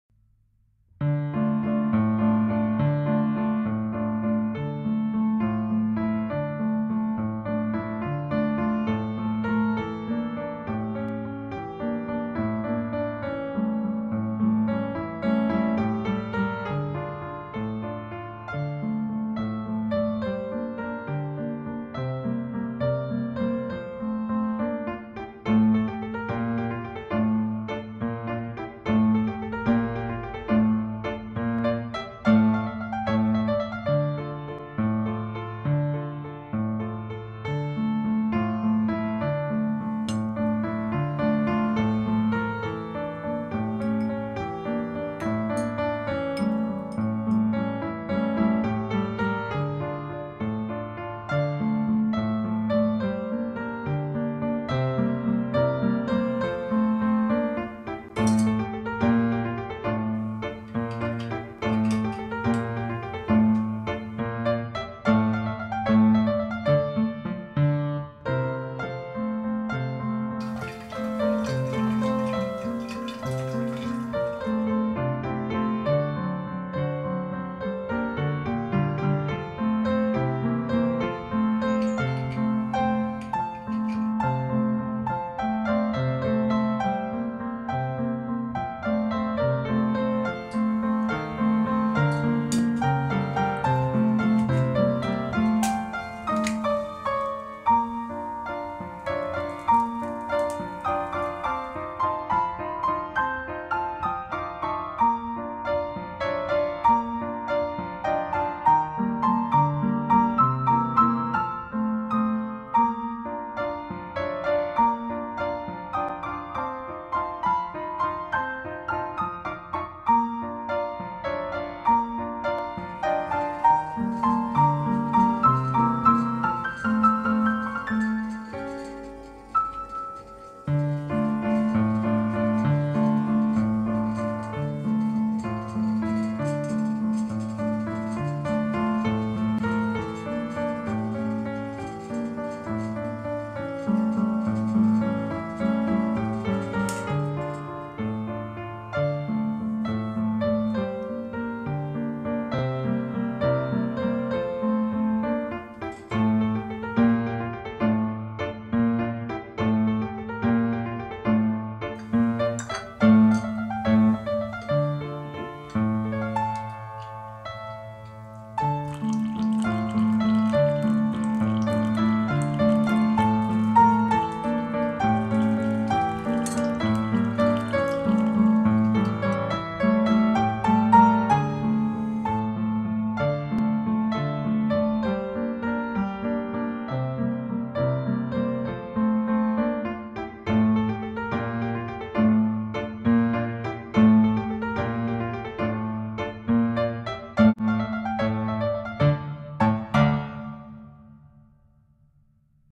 Brew coffee 19th century style with a balancing siphon
Looking for a whimsical yet scientific way to serve coffee or tea to guests with showy flair? Consider the Continental balancing siphon coffee brewer developed in the 19th century.